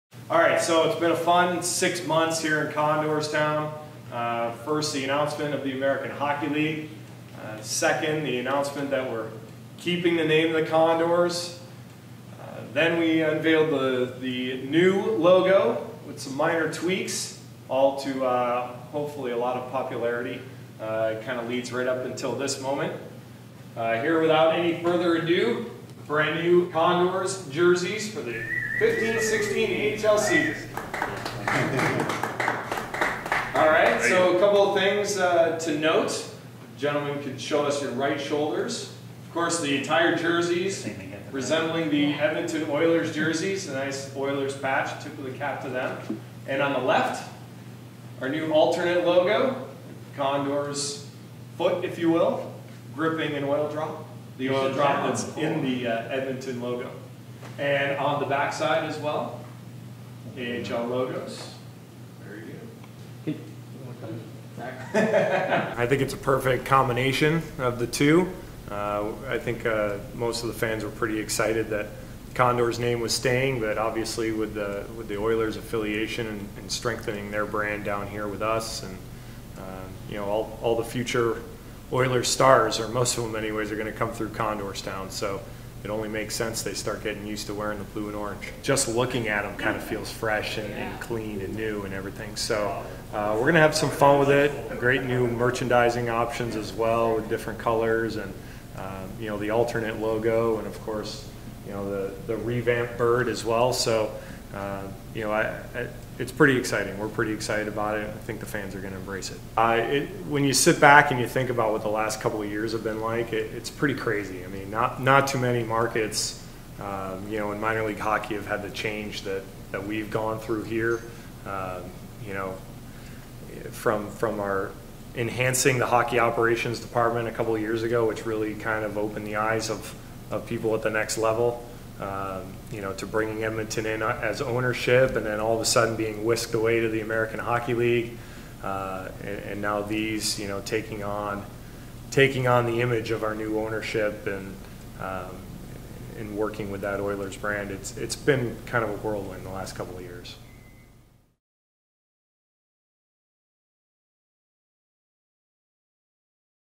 News: Bakersfield Condors Unveil New Jerseys
The Bakersfield Condors will be looking a lot like the Edmonton Oilers this fall.

That is to be expected as the Condors, heading into their first season of AHL play in the fall, are owned by the Oilers.

The new AHL jerseys were unveiled on Wednesday and with a couple of exceptions — the new tweaked Condors logo on the front being one — are pretty much the same as those of the parent club.

Read the full story on Bakersfield.com